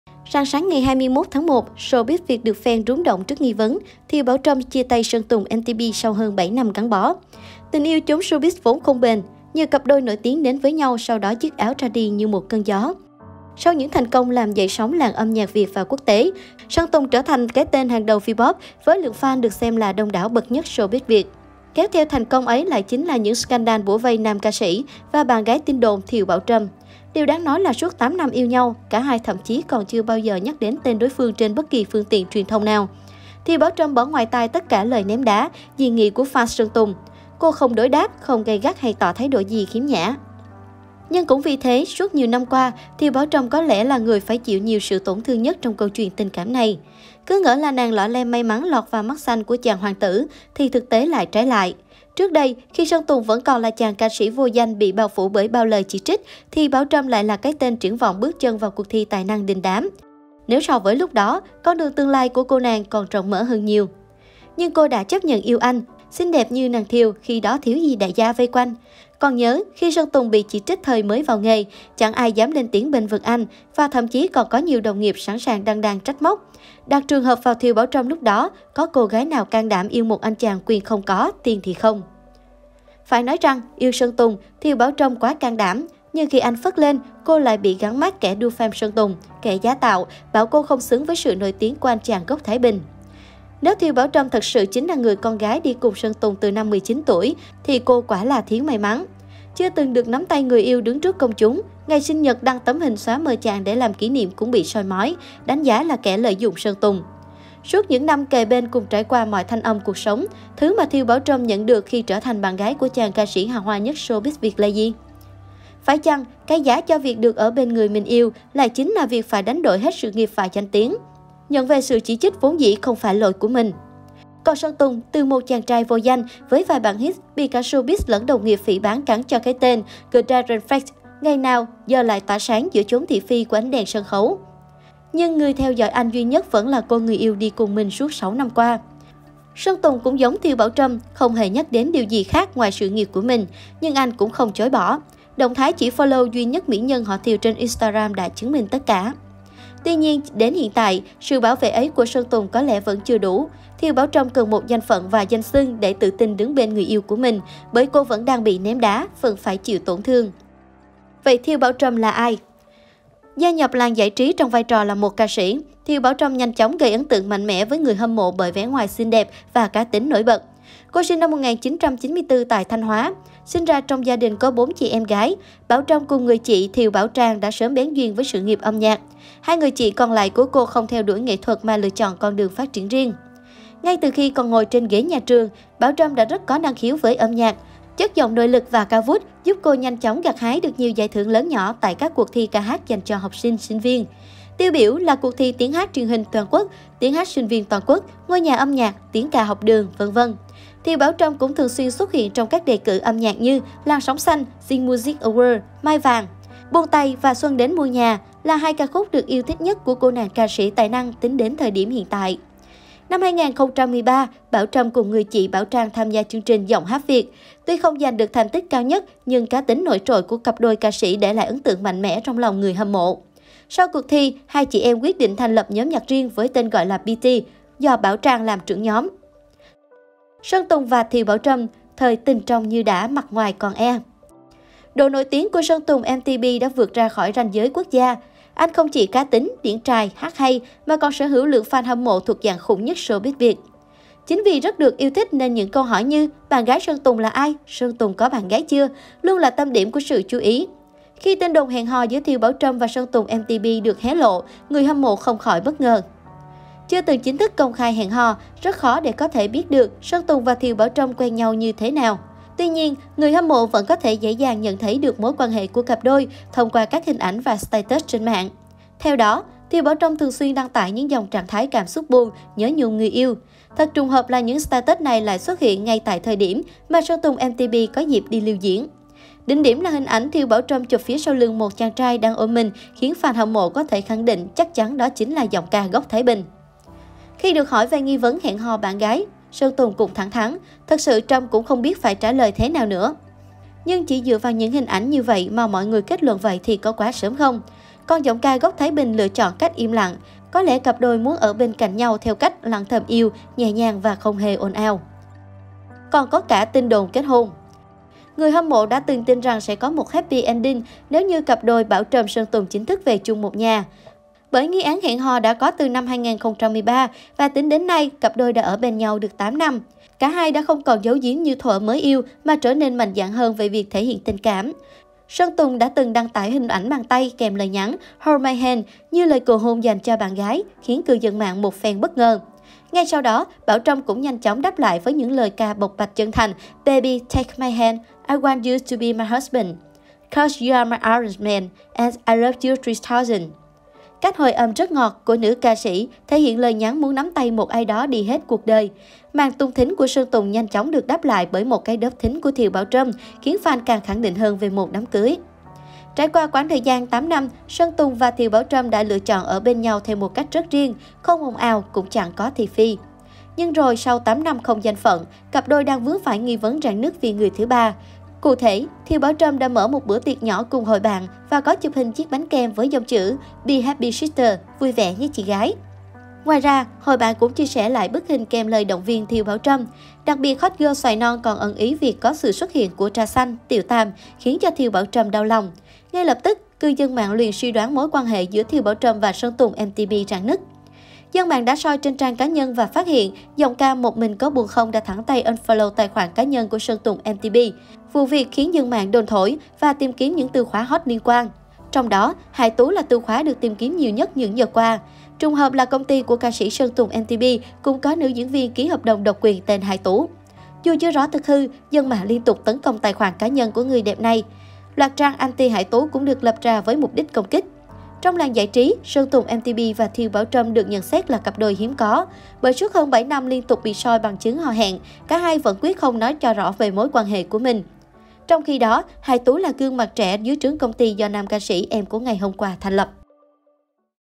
Thiều Bảo Trâm nhận được gì sau 8 năm không danh phận bên Sơn Tùng M-TP?
Rạng sáng 21/1, showbiz Việt được phen rúng động trước nghi vấn Thiều Bảo Trâm chia tay Sơn Tùng M-TP sau hơn 7 năm gắn bó. Tình yêu chốn showbiz vốn không bền, nhiều cặp đôi nổi tiếng đến với nhau sau đó “dứt áo ra đi” như một cơn gió. Sau những thành công làm dậy sóng làng âm nhạc Việt và quốc tế, Sơn Tùng trở thành cái tên hàng đầu Vpop với lượng fan được xem là đông đảo bậc nhất showbiz Việt. Kéo theo thành công ấy lại chính là những scandal bủa vây nam ca sĩ và "bạn gái tin đồn" Thiều Bảo Trâm. Điều đáng nói là suốt 8 năm yêu nhau, cả hai thậm chí còn chưa bao giờ nhắc đến tên đối phương trên bất kỳ phương tiện truyền thông nào. Thiều Bảo Trâm bỏ ngoài tai tất cả lời ném đá, dị nghị của fans Sơn Tùng, cô không đối đáp, không gay gắt hay tỏ thái độ gì khiếm nhã.
Nhưng cũng vì thế, suốt nhiều năm qua, Thiều Bảo Trâm có lẽ là người phải chịu nhiều sự tổn thương nhất trong câu chuyện tình cảm này. Cứ ngỡ là nàng Lọ Lem may mắn lọt vào mắt xanh của chàng hoàng tử thì thực tế lại trái lại. Trước đây, khi Sơn Tùng vẫn còn là chàng ca sĩ vô danh bị bao phủ bởi bao lời chỉ trích thì Bảo Trâm lại là cái tên triển vọng bước chân từ cuộc thi tài năng đình đám. Nếu so với lúc đó, con đường tương lai của cô nàng còn rộng mở hơn nhiều.